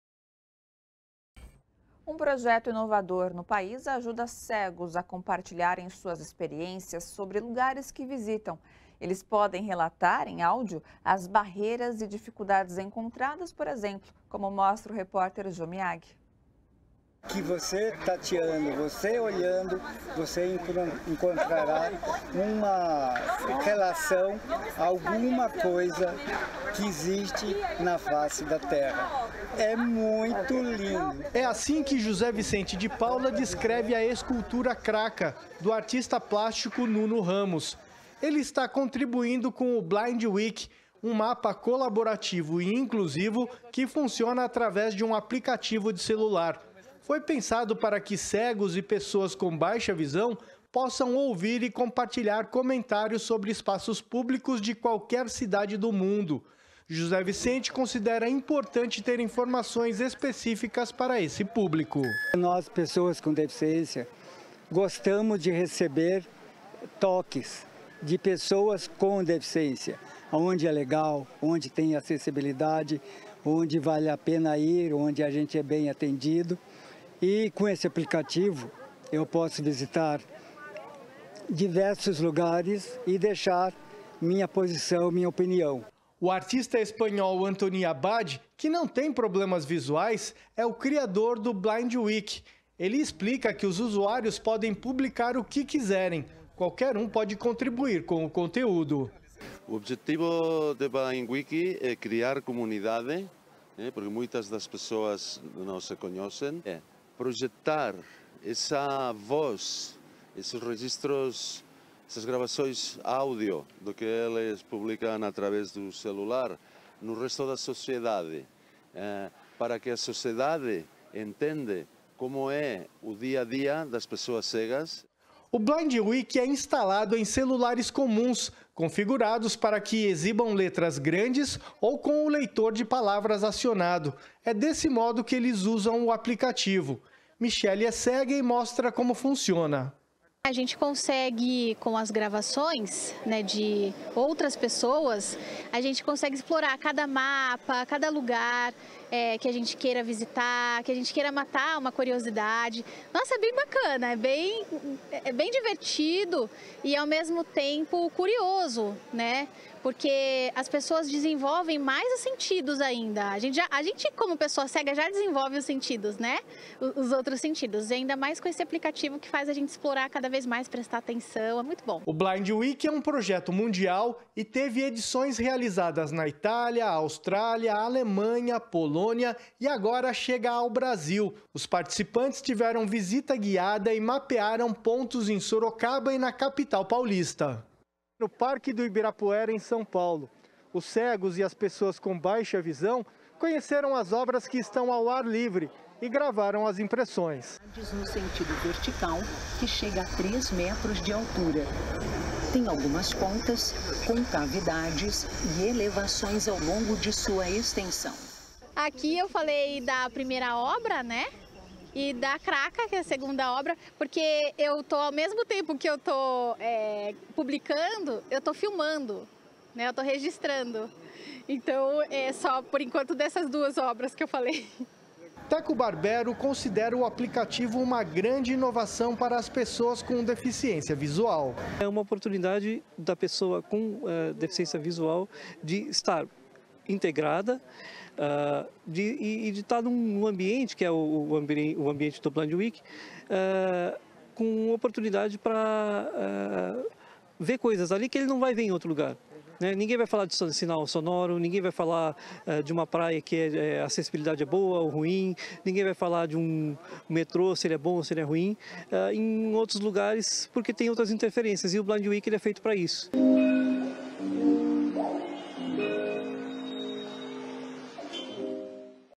2022 São Paulo *POR · TV dos Trabalhadores
Projeto ajuda cegos a compartilharem experiências sobre lugares que visitam